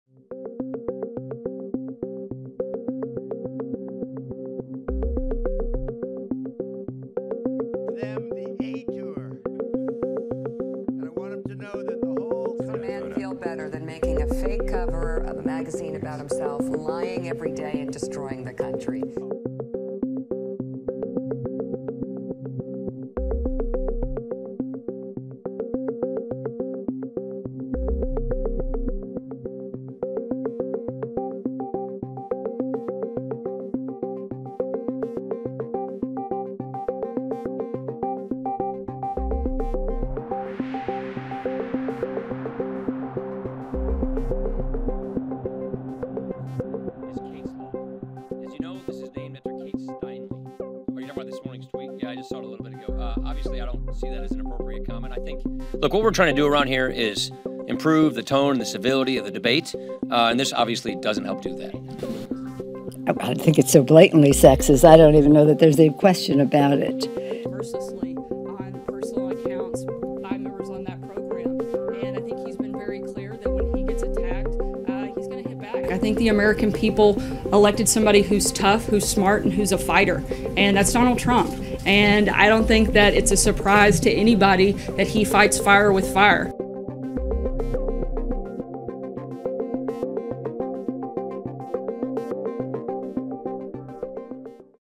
Trump mocks show host, says “she was bleeding badly from a face-lift”
President Donald Trump on Thursday ridiculed the brains, looks and temperament of a female cable television host whose show he says he has stopped watching. His latest crude broadside against a woman's appearance set off a storm of protest from Republicans and Democrats alike and did nothing to advance his struggling policy agenda. In a series of tweets morning tweets, the president went after Mika Brzezinski and Joe Scarborough, who have criticized Trump on their MSNBC show "Morning Joe."